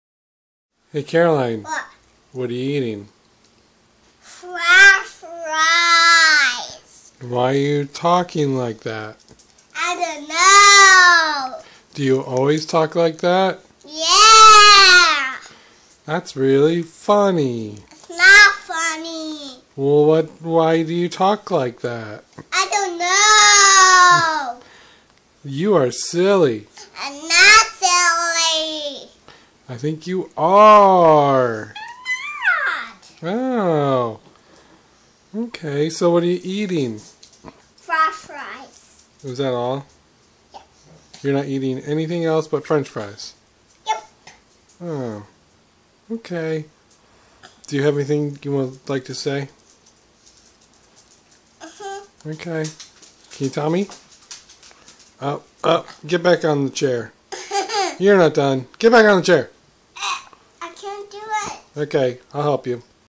Caroline eats French fries sarcastically
Drama queen